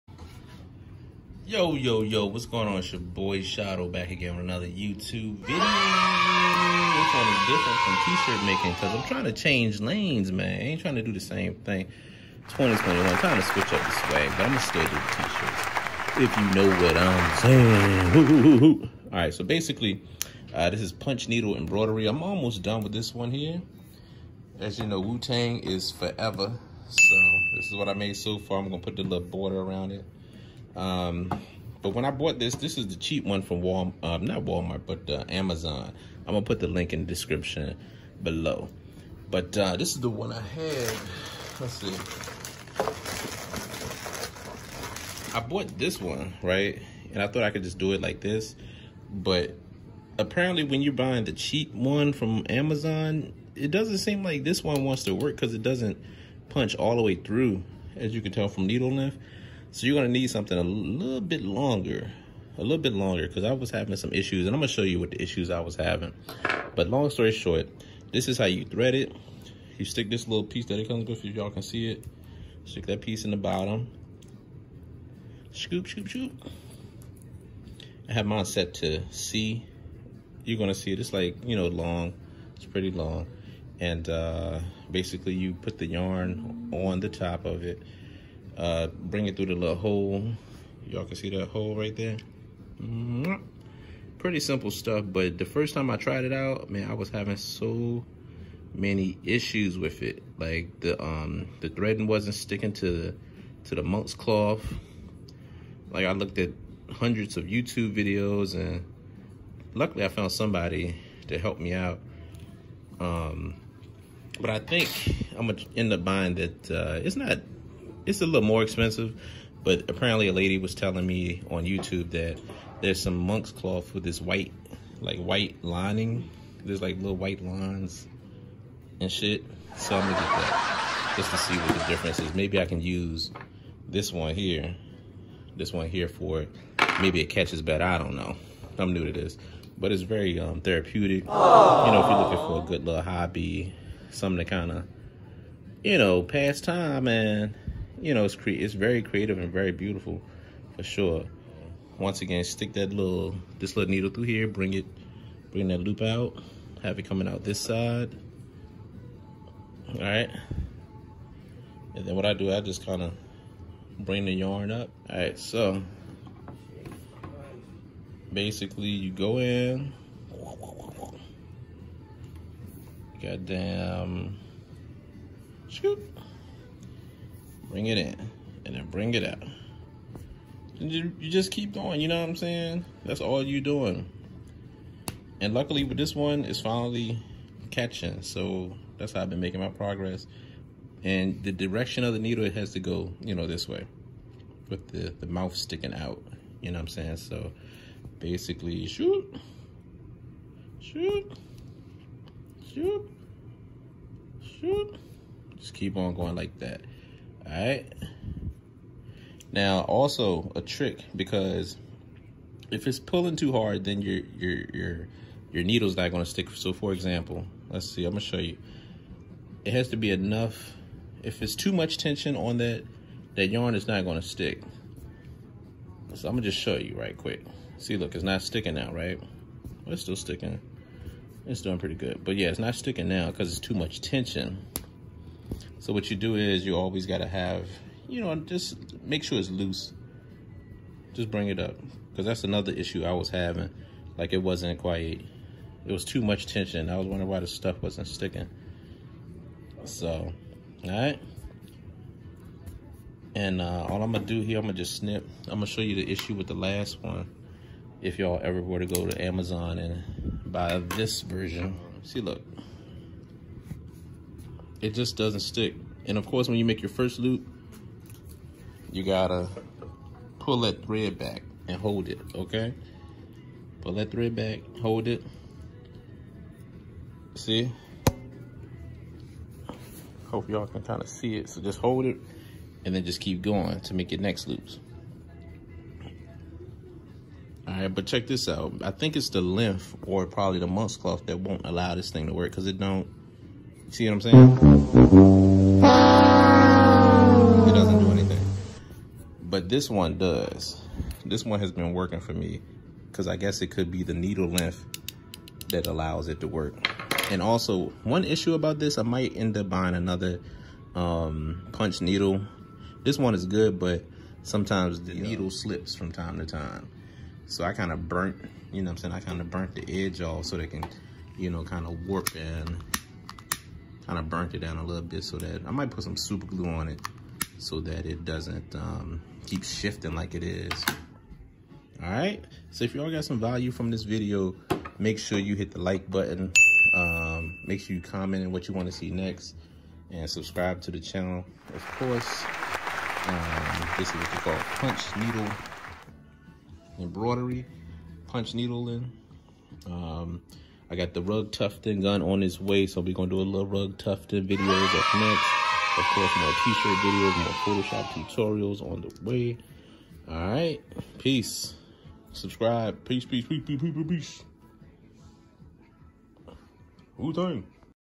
How to Punch Needle for Beginners | Punch Needle Embroidery Amazon Kit
I am a beginner but I finally figured out how to use the kit from Amazon!
I hope this helps someone out! This hobby is very relaxing, affordable and I also have the rug tufting gun on the way so I will be making videos on that soon!

The kit purchased was from amazon

Frame used